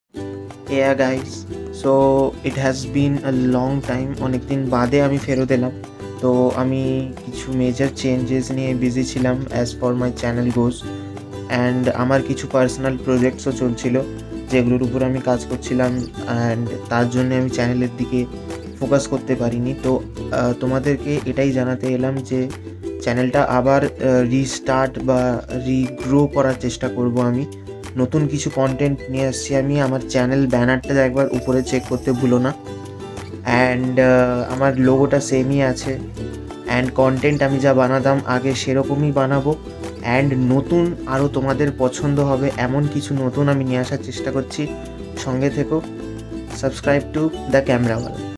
New Gifts 💥 Mobile photography tips for beginners || The Kamerawala
Thank you পি. আর. স্টুডিওস , NIRI 9 and Junmoni Devi Khaund Ma'am. Thanks to the Entire Team and Crew of "Jodi Tumi Jante"

 Releasing On the 18th of Jan.

This is a Short introductory video about the New Restart of my channel which was discontinued due to some issues since May 21.

I'm bringing new Content as promised. and Today a Short clip is coming which announce the big Surprise 💥🌻